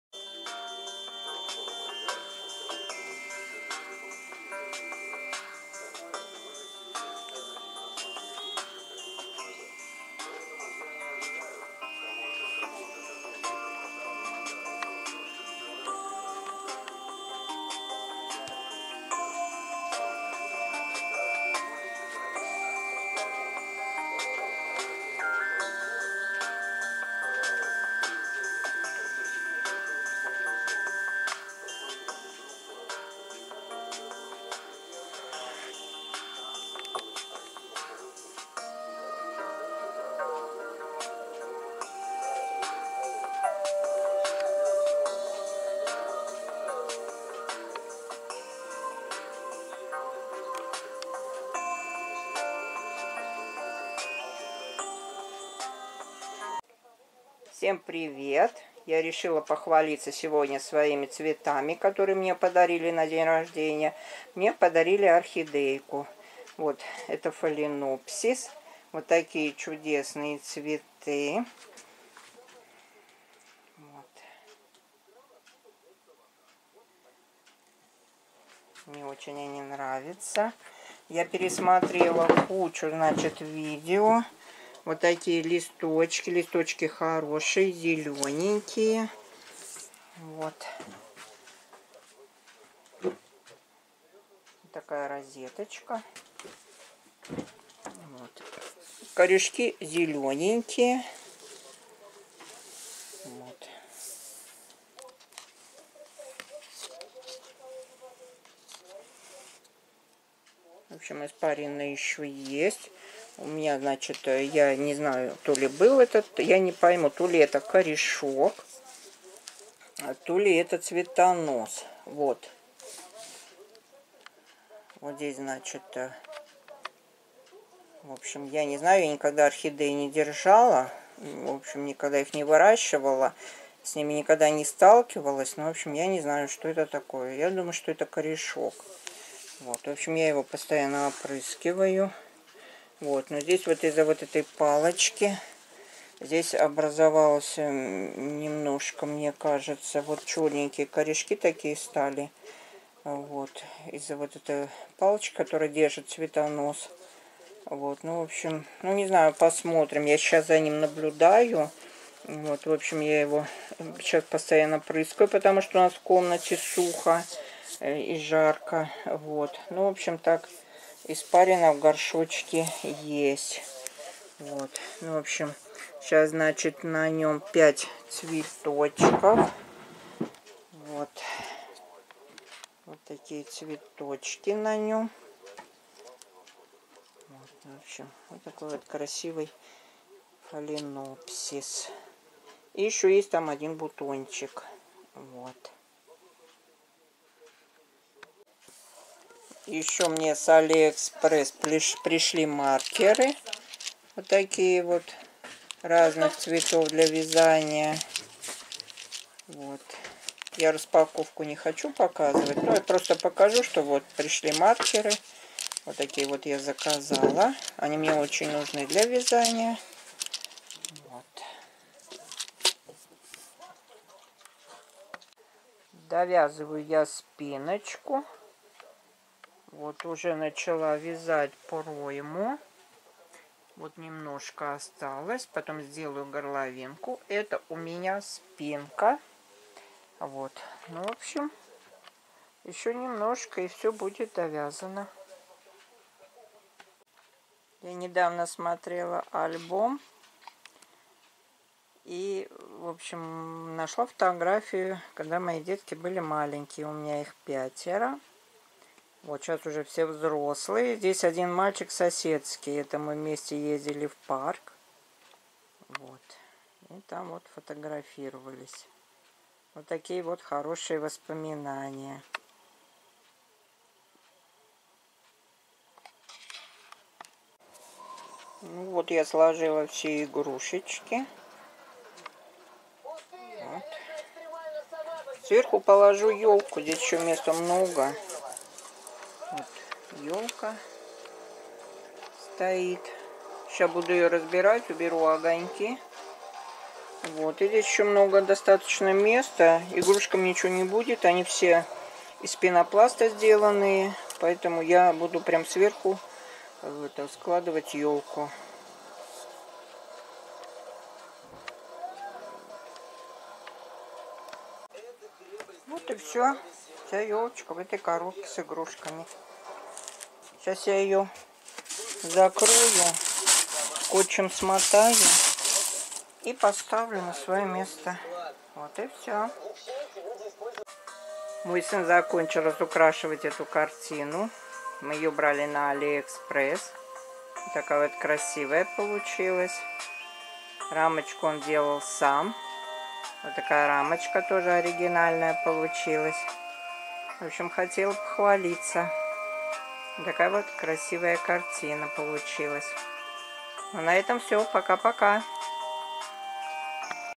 Мой первый фаленопсис . Маркеры для вязания с алиэкспресс.
И еще вяжу платье.Сложила в коробку  елку. Похвалилась картиной.